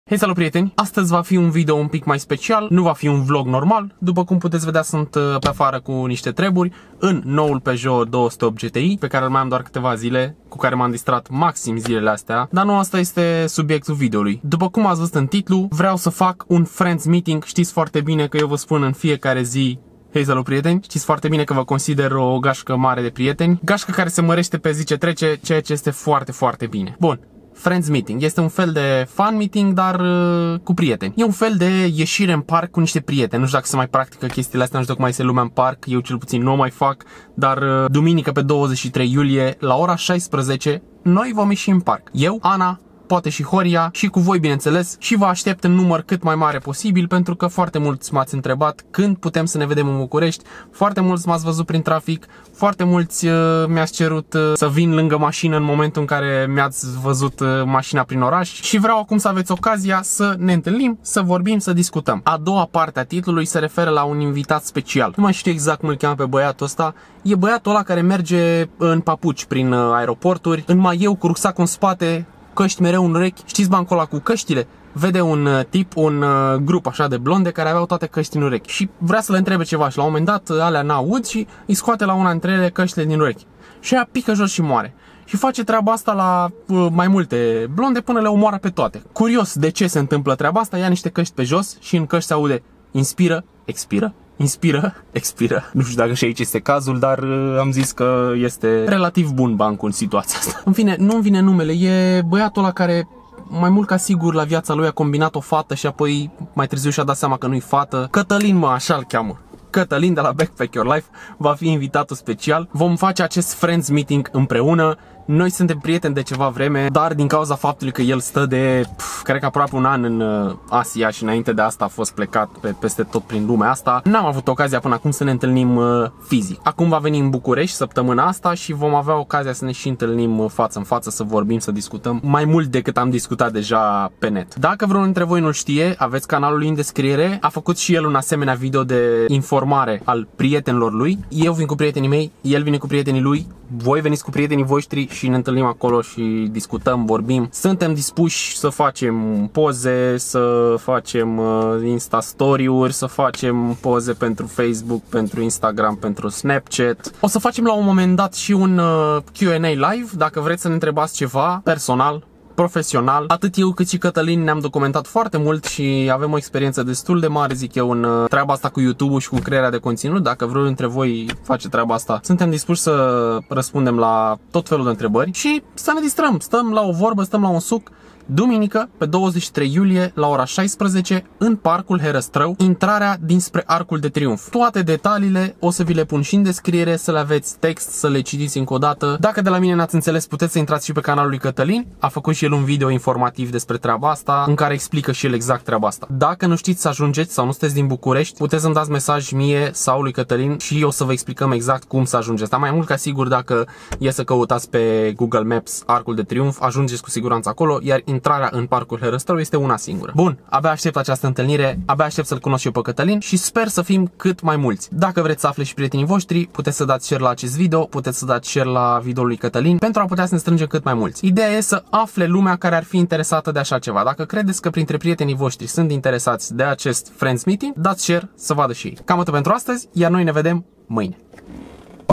FRIENDS MEETING BUCURESTI! (+ INVITAT SPECIAL) - Vlog 568
Intalnirea va avea loc Duminica, 23 iulie 2017, ora 16:00. Ne vom vedea in Parcul Herastrau, intrarea de la Arcul de Triumf!
Va astept in numar cat mai mare! :)